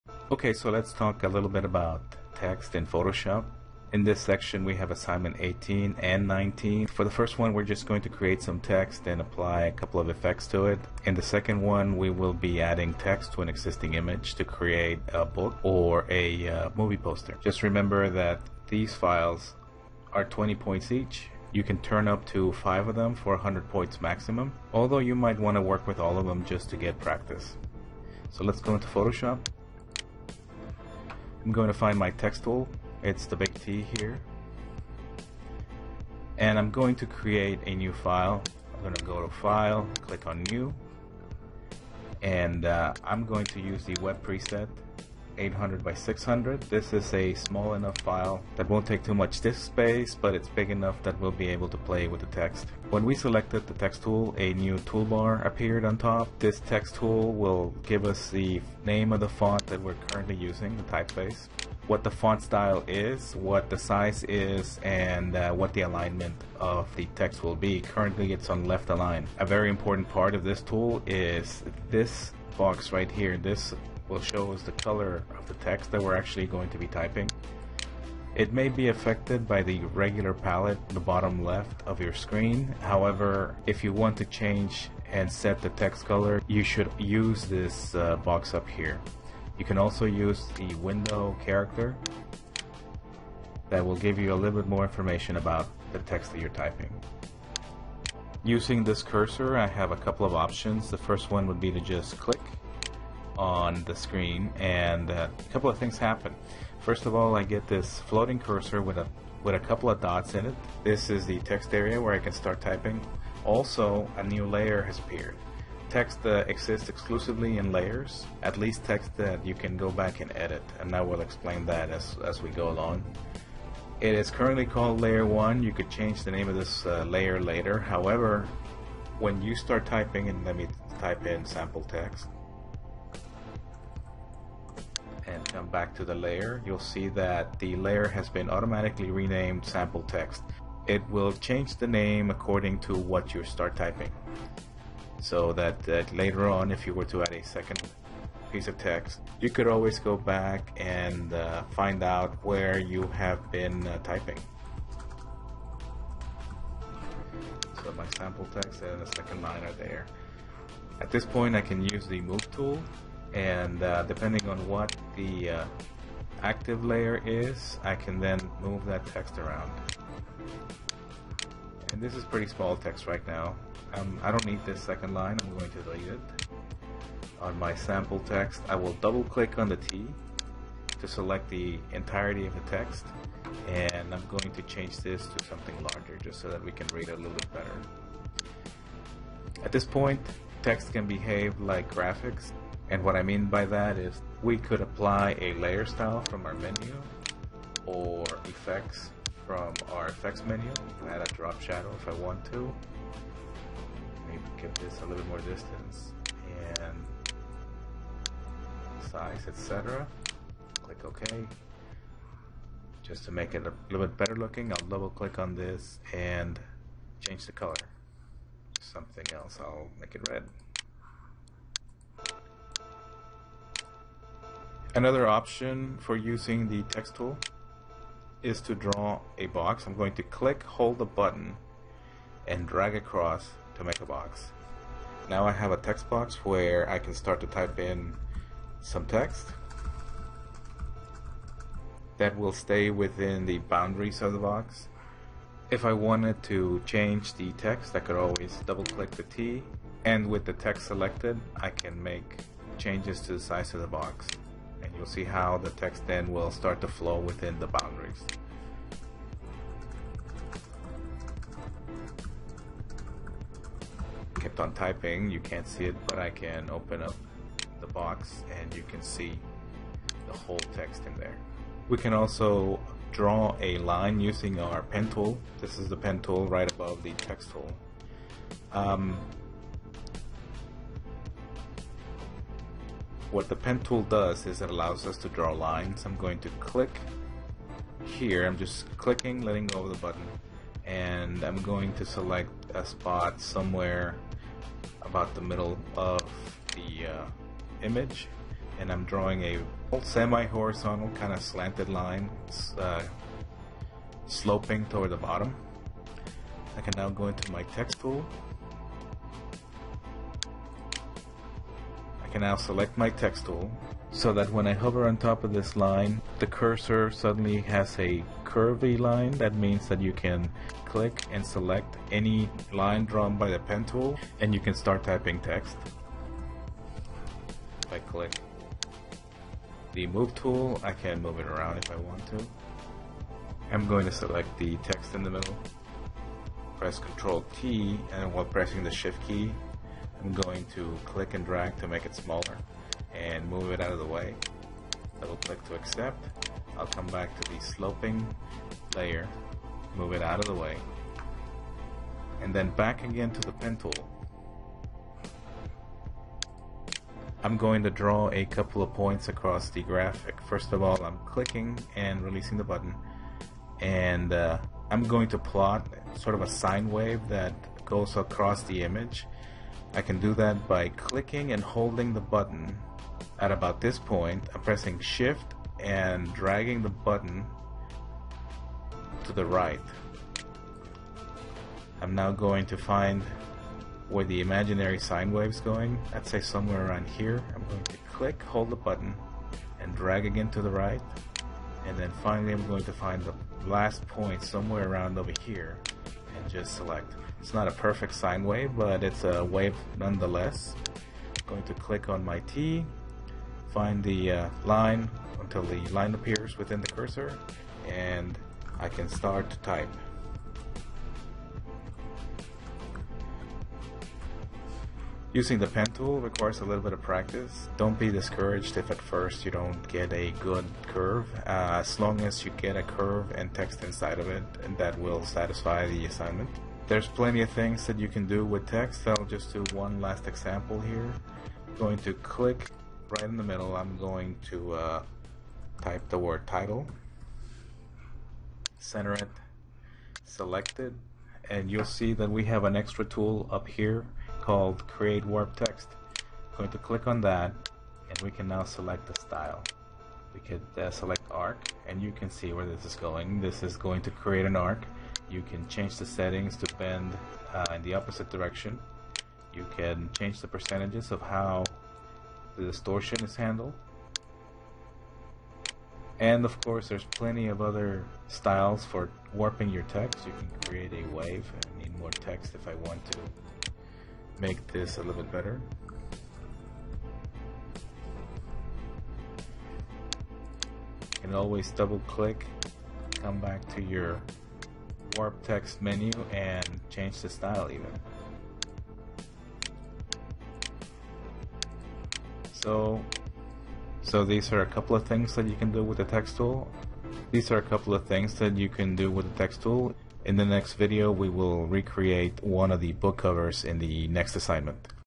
Using the Photoshop Text Tool
Use the text tool to produce text in Photoshop layers and apply effects and styles to it.